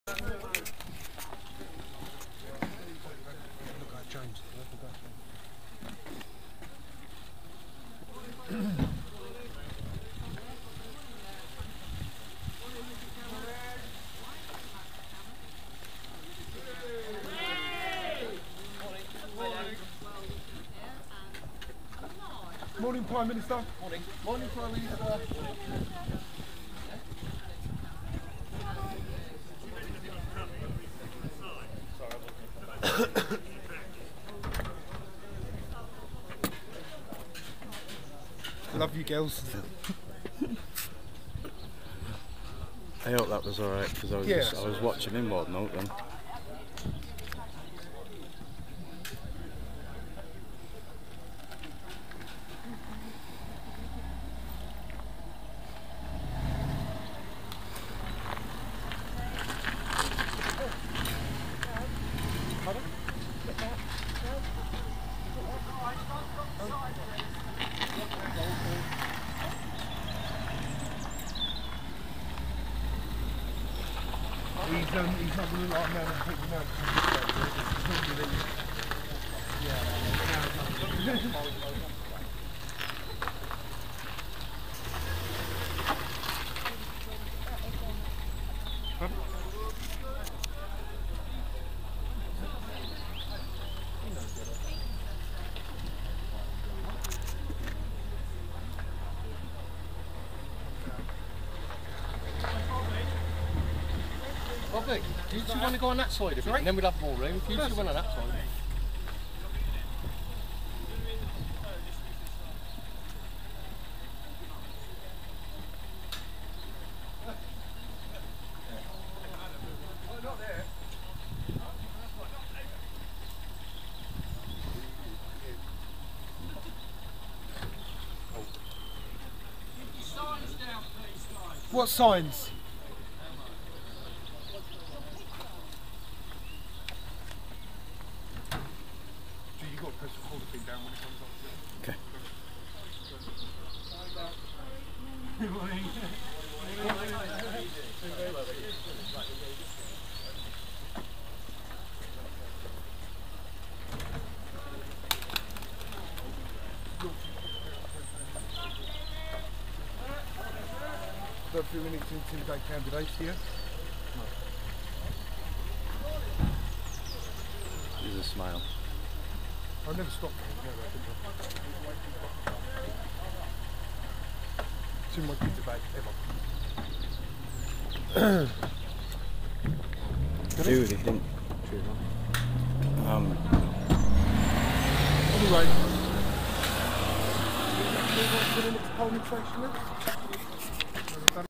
David Cameron gets photo bombed
fathers rights campaigners Darryn Thurtle, Carol Wheeler, Bobby Smith and James Dennis photo bomb David Camerons trip to the polling station, general election 2015 Witney Oxfordshire England.